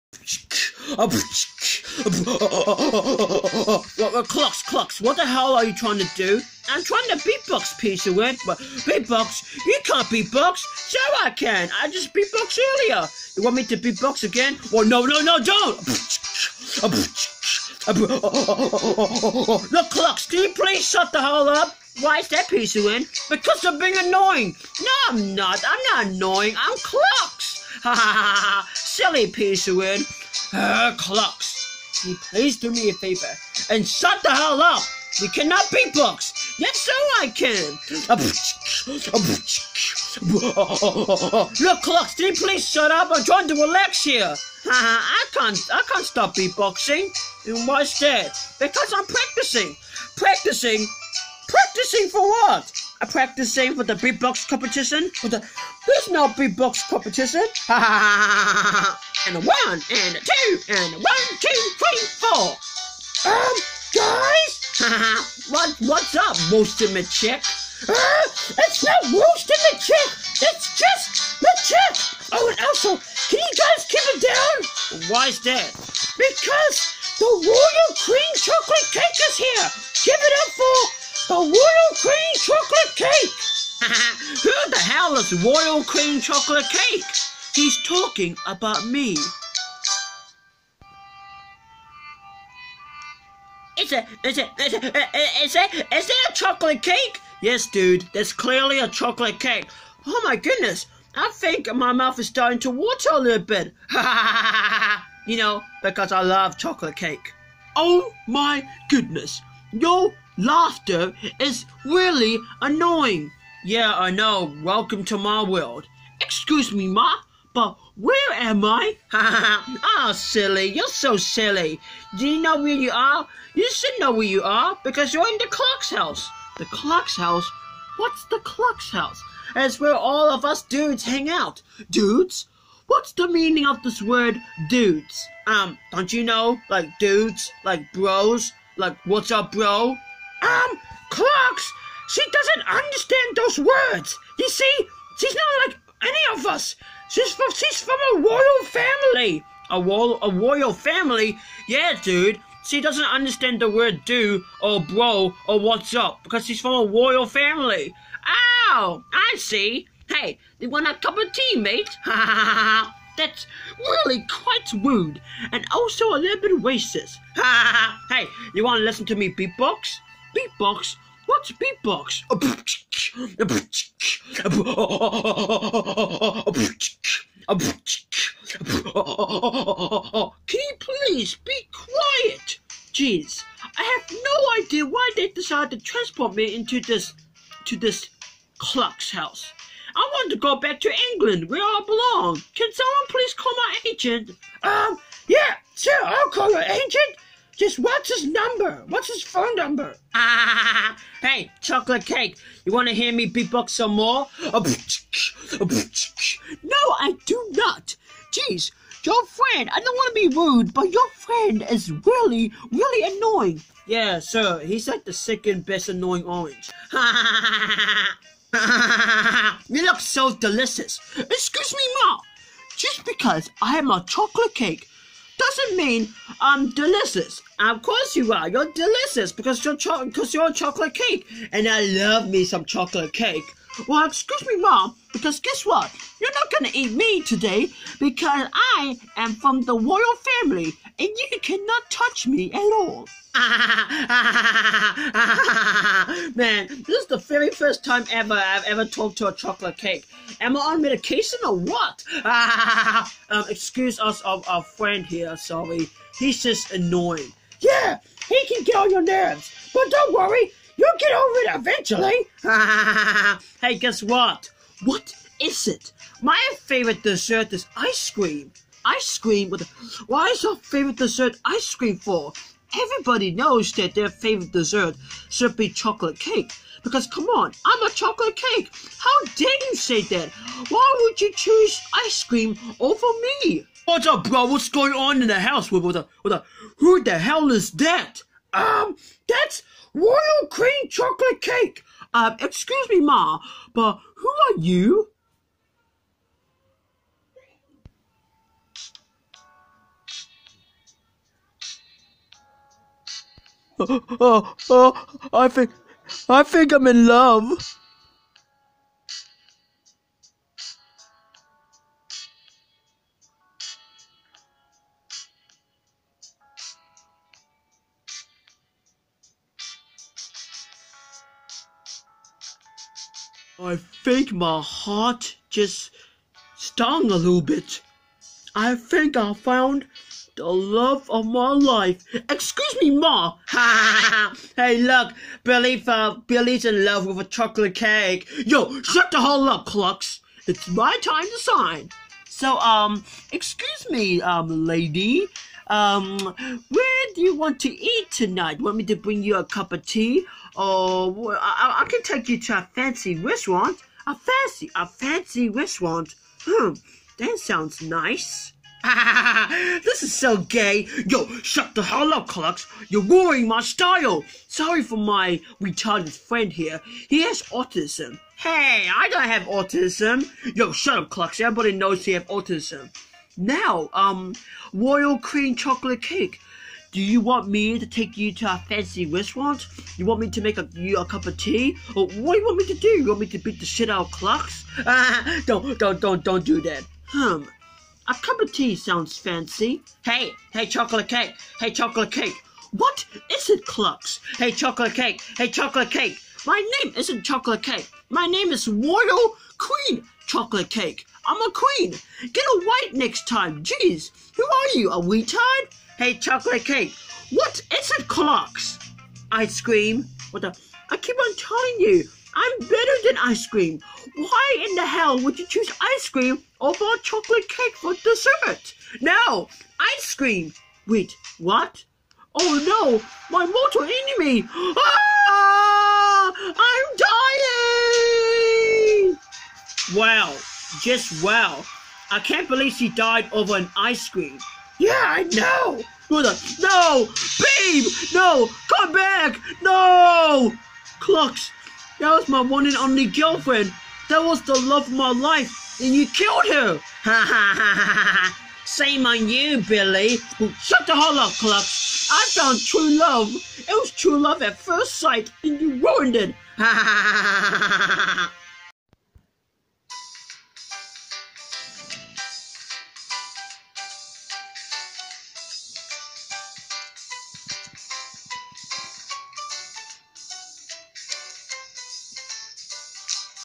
Clucks and Friends: Queen Chocolate Cake!
A royal queen chocolate cake comes and visits the Clucks house!!!